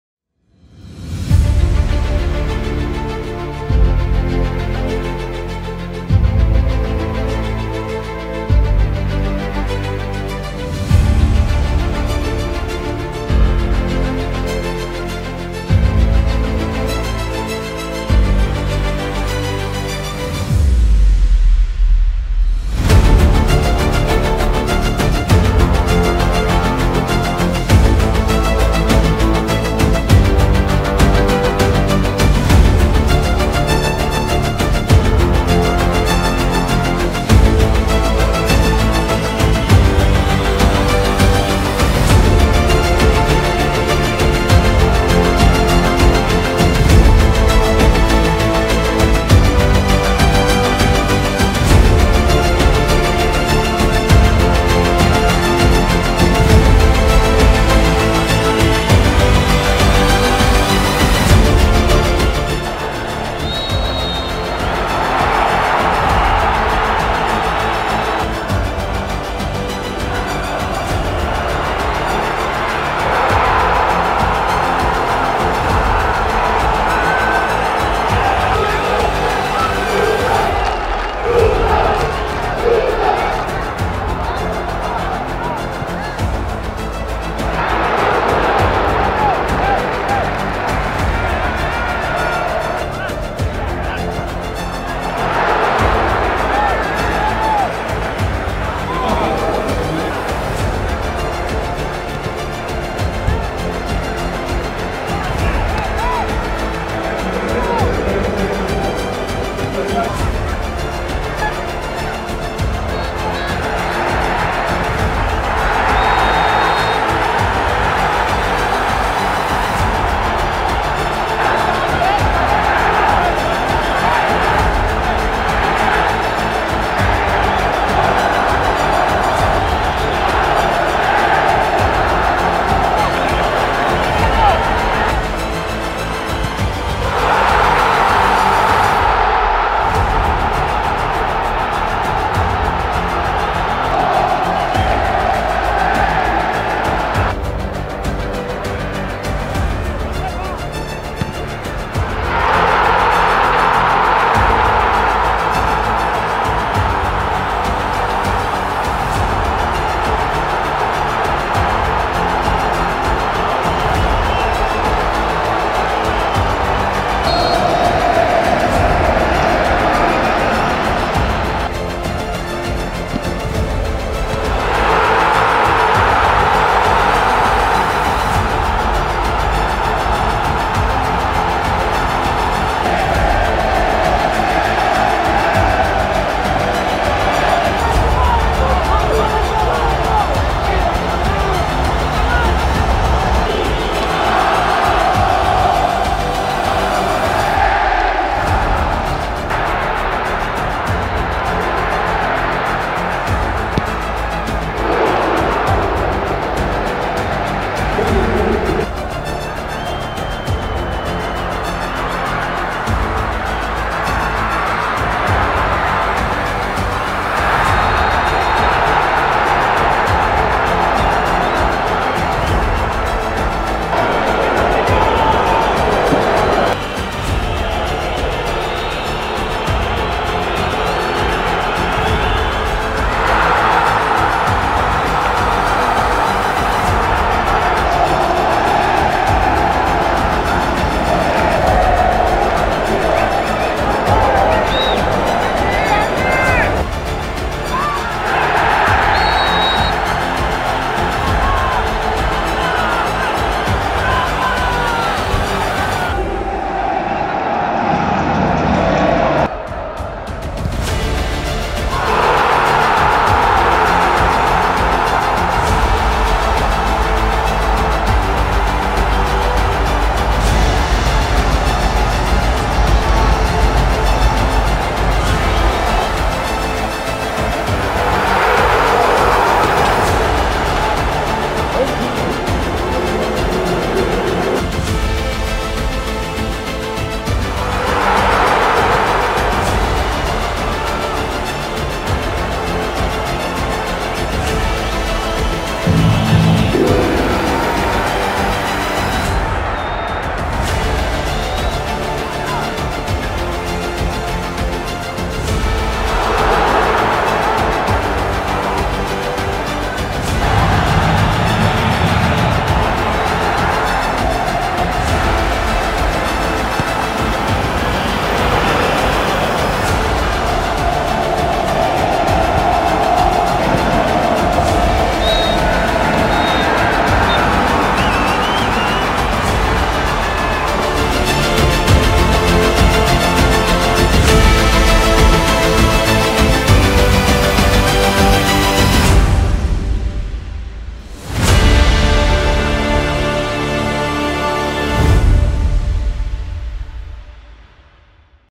Color América vs Pachuca | Liga BBVA MX - Guard1anes 2021 | 4K
¡¡¡Fue un partidazo!!!

Pachuca y América disputaron el último boleto a las Semifinales del

Todos los detalles de este emocionante encuentro los recopilamos aquí. Chécalos.